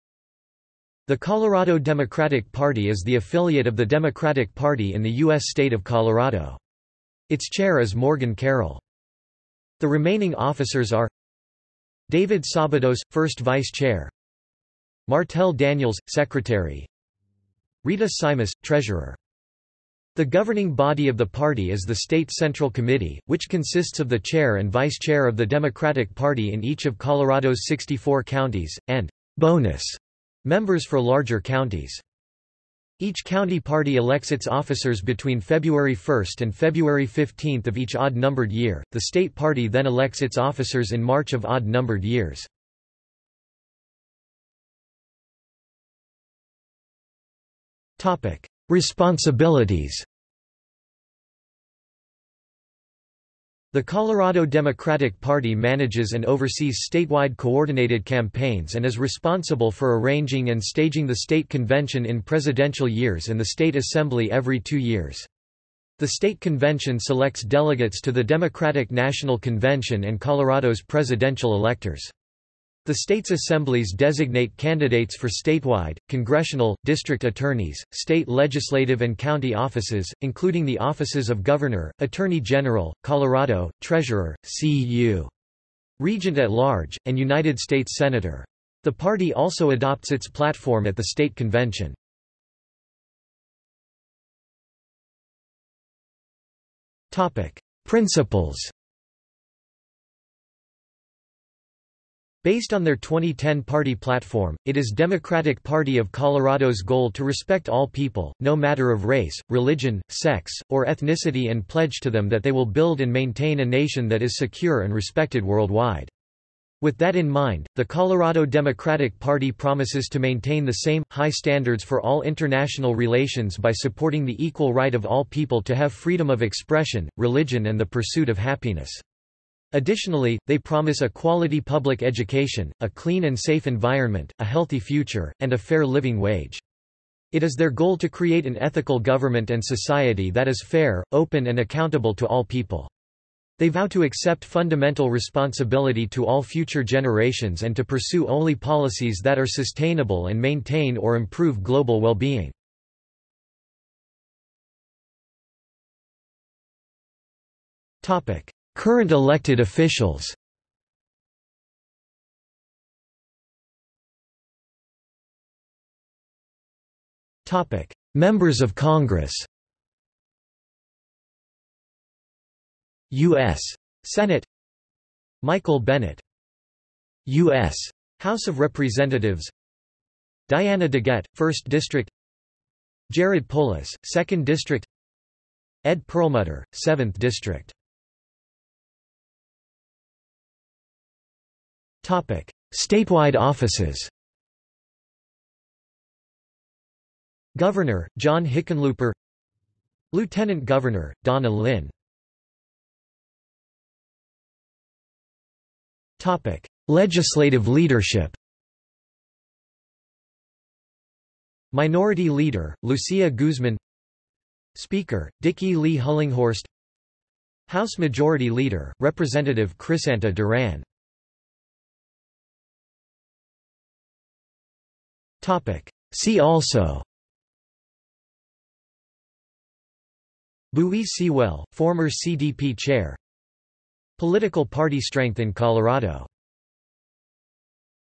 Colorado Democratic Party | Wikipedia audio article
This is an audio version of the Wikipedia Article:
Colorado Democratic Party

00:00:44 1 Responsibilities
00:01:33 2 Principles
00:02:34 3 Current elected officials
00:02:44 3.1 Members of Congress
00:02:53 3.1.1 U.S. Senate
00:02:57 3.1.2 U.S. House of Representatives
00:03:08 3.2 Statewide offices
00:03:22 3.3 Legislative leadership
00:03:41 4 See also

- Socrates

SUMMARY
The remaining officers are:
David Sabados - First Vice Chair;
Martelle Daniels - Secretary;
Rita Simas - Treasurer.
The governing body of the party is the State Central Committee, which consists of the Chair and Vice-Chair of the Democratic Party in each of Colorado's 64 counties, and "bonus" members for larger counties.
Each county party elects its officers between February 1 and February 15 of each odd-numbered year; the state party then elects its officers in March of odd-numbered years.